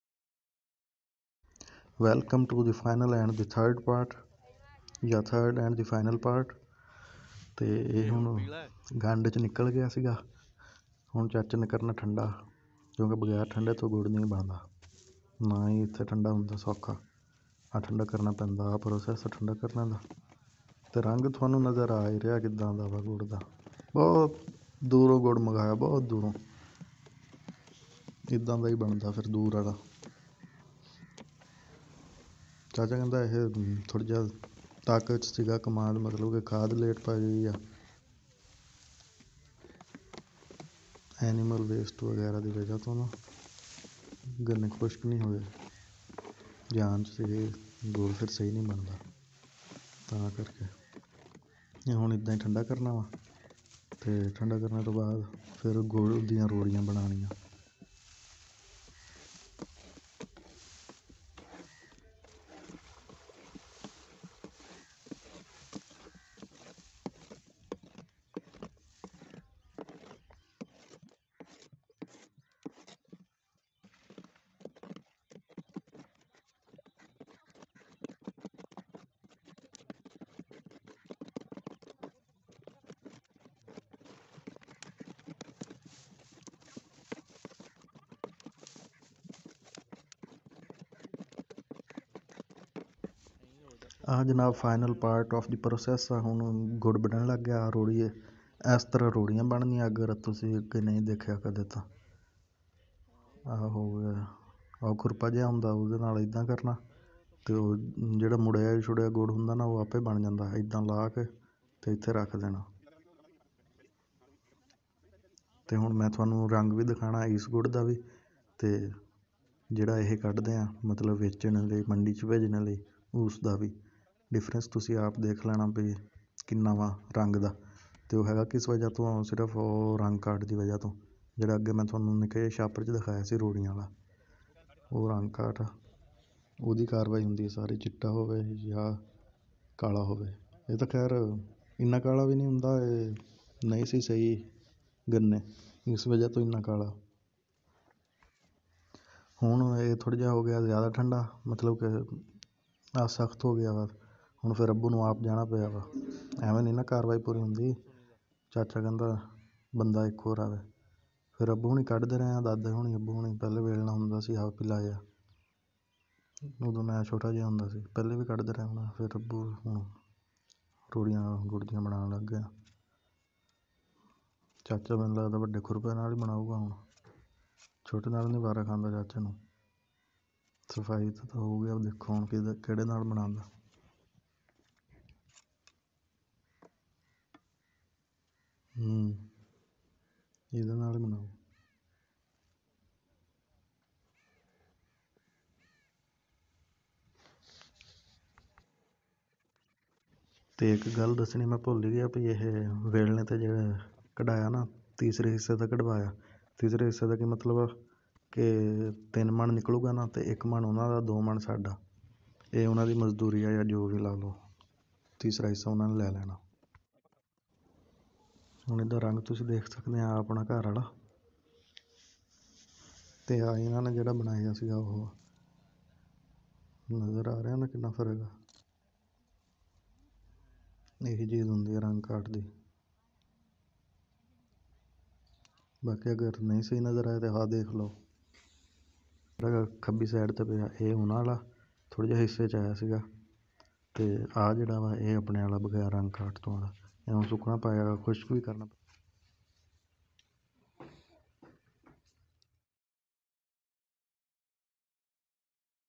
Gurr Kesy Banaya Jata Hai. | Final Part. | How Is Jaggery Made Complete Process.
گڑ کیسے بنایا جاتا ہے؟ دیکھیں اس ویڈیو میں۔
آج ہم نے گھر استعمال کے لیے گڑ بنوایا۔

Gud kis Tarha Banaya Jata ha. kia kia process krna parta hai is video mn dekhy ye pehla part hai.